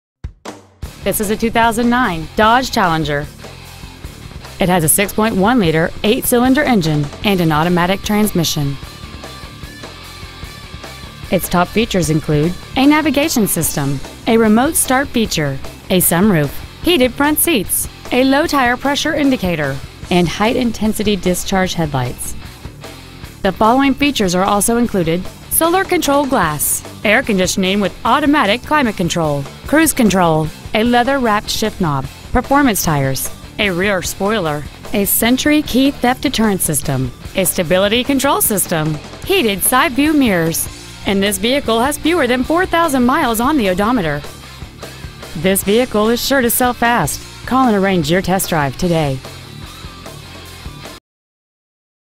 2009 Dodge Challenger Stockton IL 61085
We have been honored to serve the Stockton IL area , we promise that your experience at our dealership will exceed your expectations !

 Year : 2009
 Make : Dodge
 Model : Challenger
 Engine : 6.1L Hemi V8 engine
 Trans . : Automatic
 Exterior : Orange
 Miles : 3,032
 Interior : Other

 501 E. North Ave.
 Stockton , IL 61085